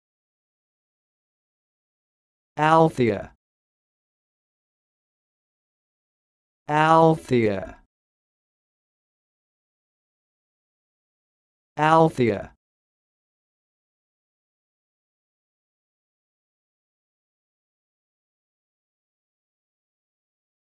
Learn how to say this word: "Althea"
This is the definition of "Althea":
n.  A genus of plants of the Mallow family. It includes the
   officinal marsh mallow, and the garden hollyhocks.